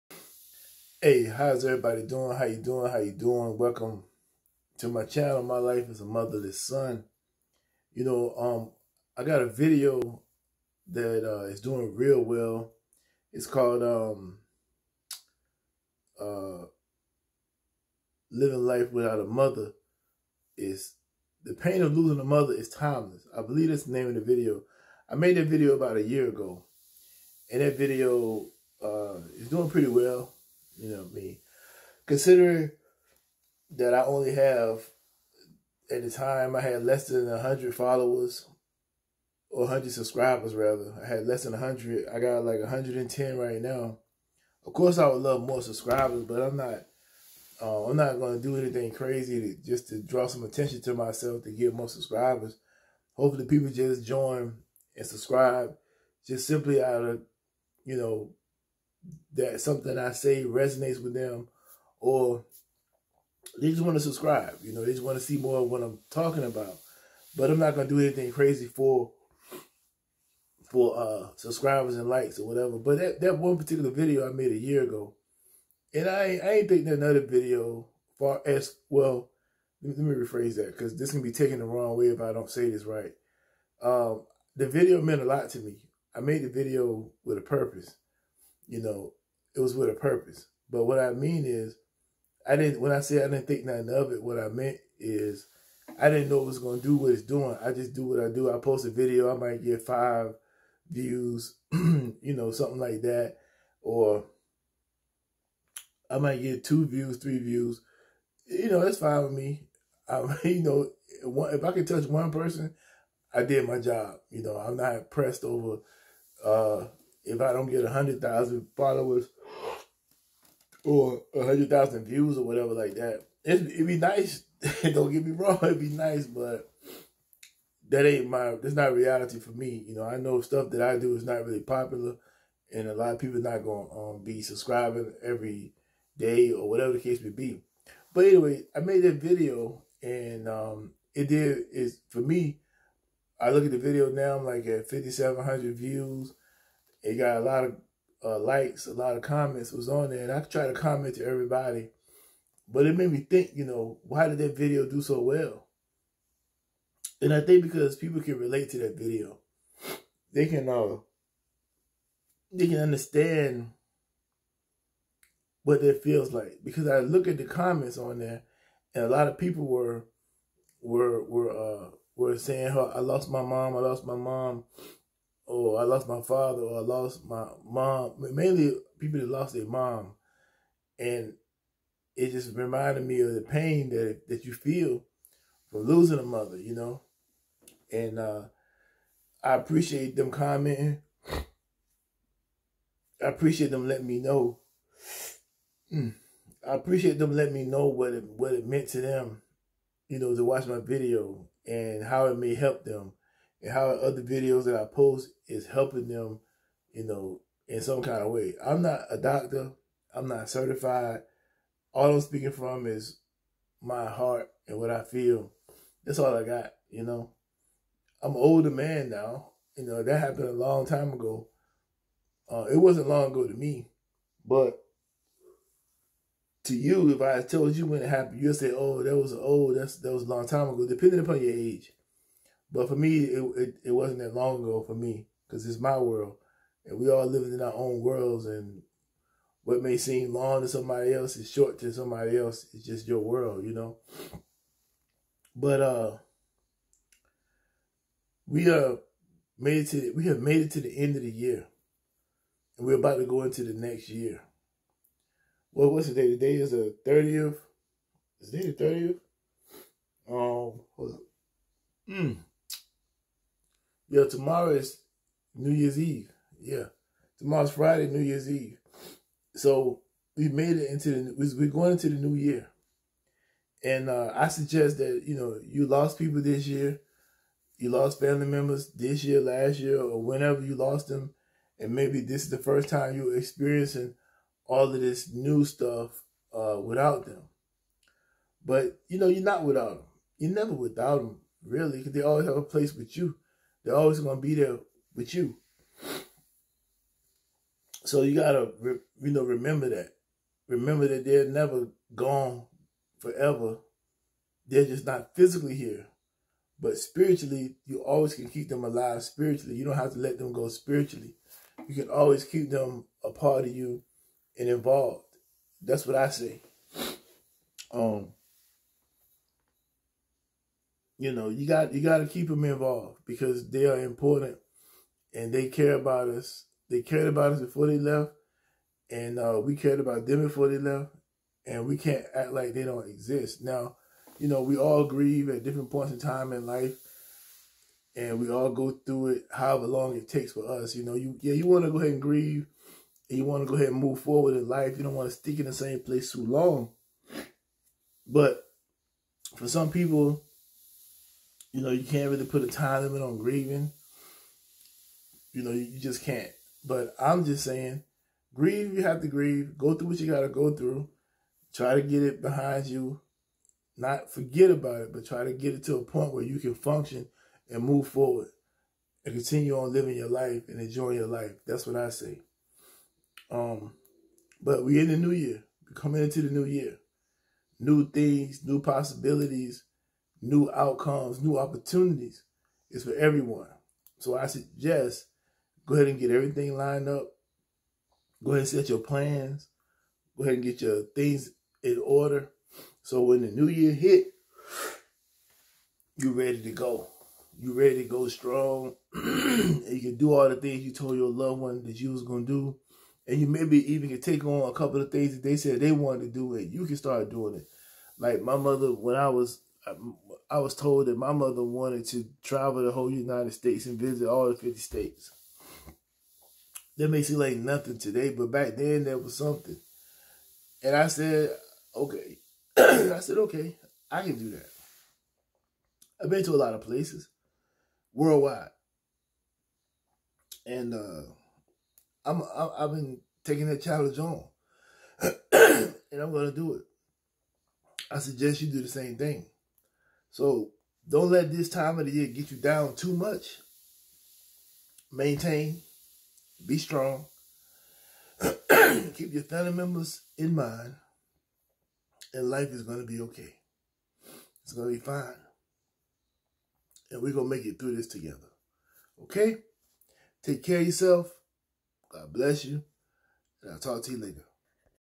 Moving into the New Year without your love ones.
We are moving into the New Year without out love ones. We can do this and we will be ok.